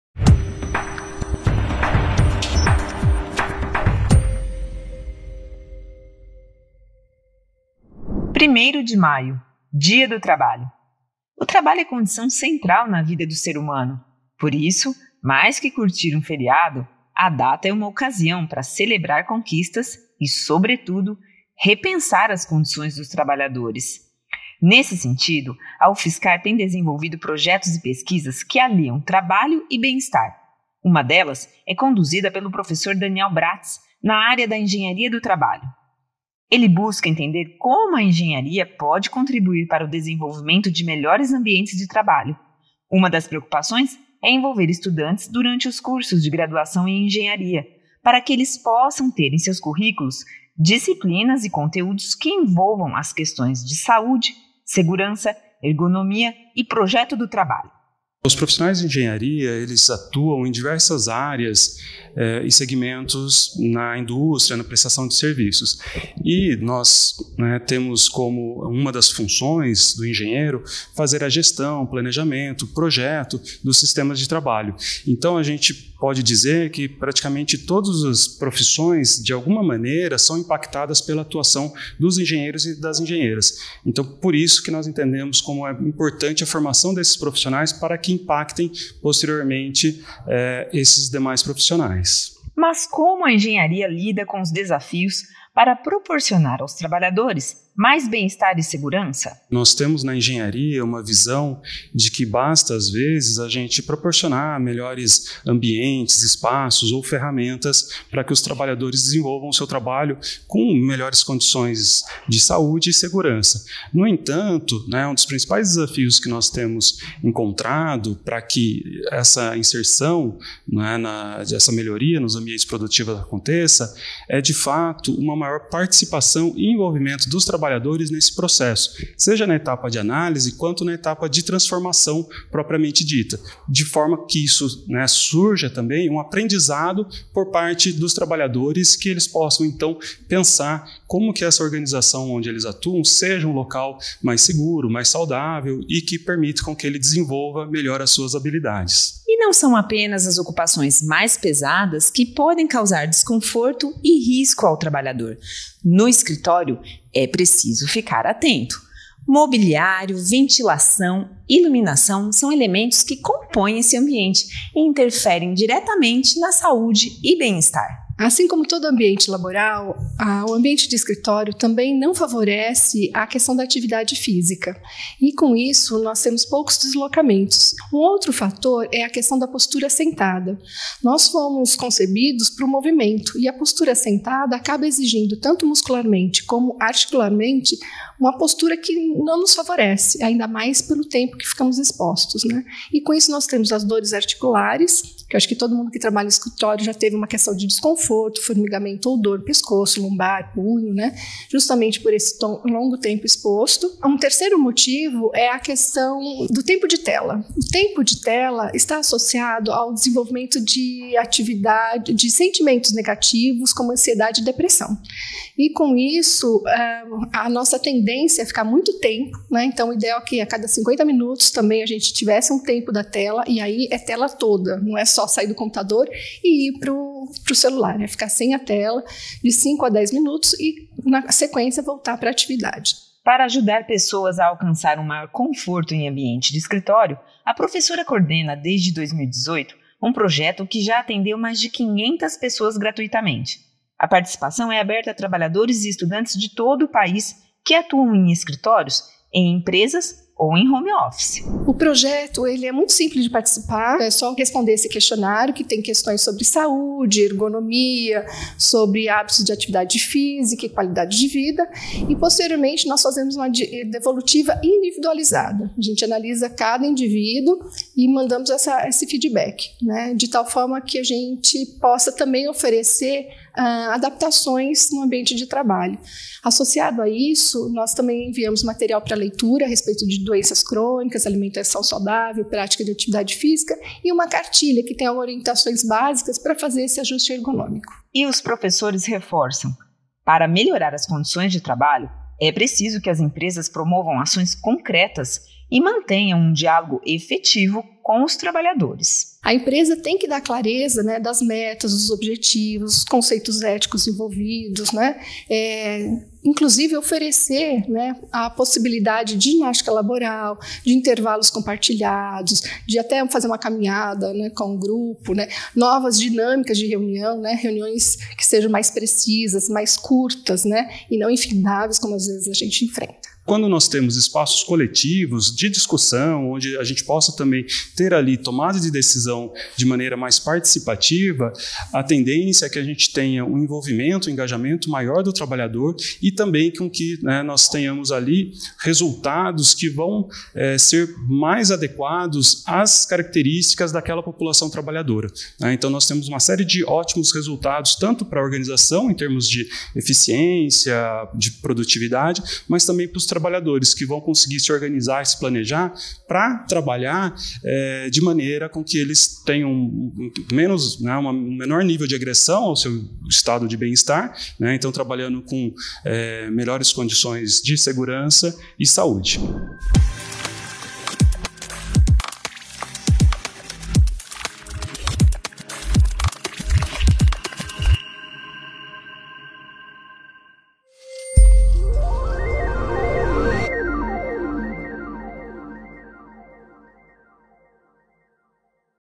Qualidade de vida para o trabalhador é foco de projetos da UFSCar
Em matéria que celebra o Dia do Trabalho, Daniel Braatz, professor do Departamento de Engenharia de Produção (DEP) fala como a Engenharia pode afetar as demais ocupações laborais. Já a professora Claudia Aparecida Stefane, do Departamento de Medicina (DMed), convida para participar de seu projeto para melhorar as condições de trabalho em escritórios. Os professores ainda dão dicas e reforçam como empresas e trabalhadores devem caminhar juntos nas decisões.